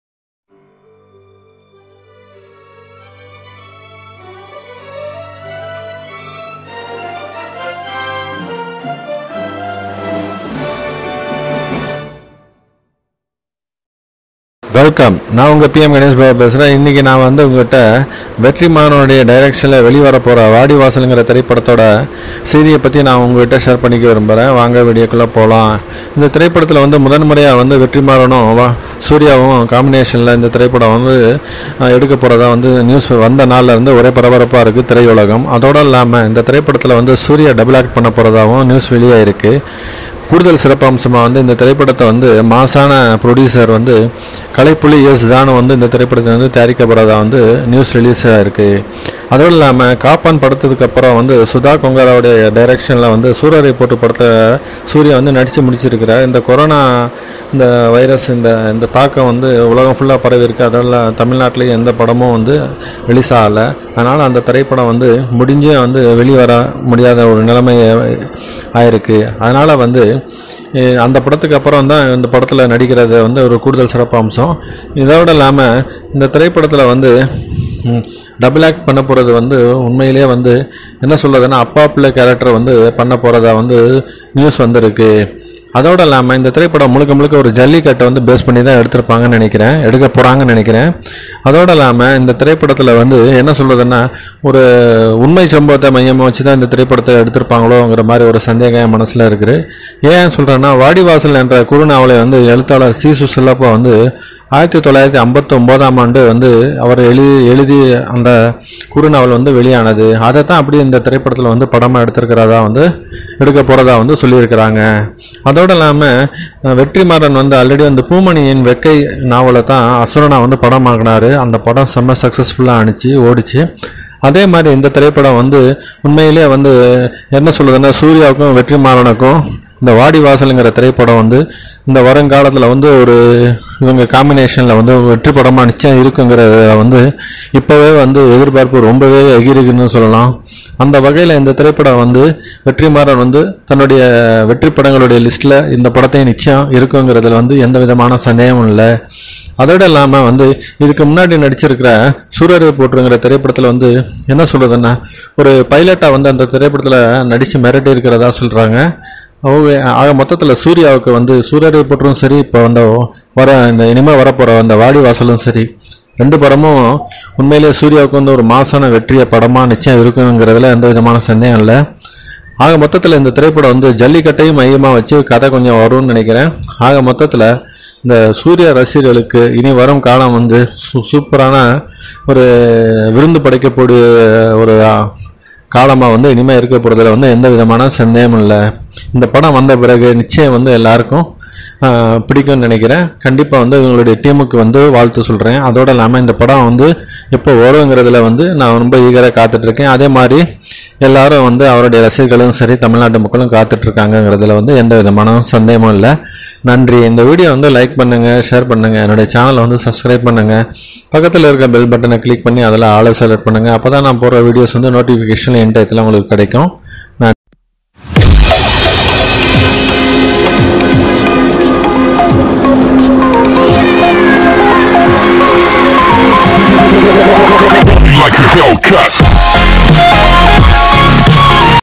vetrimarans vadivasal movie news review by pmganeshbabu
directed by   vetrimaran

acted by   suriya  double action  father and son   1st time comination with vetrimaran

produced by   kalaipuli dhanu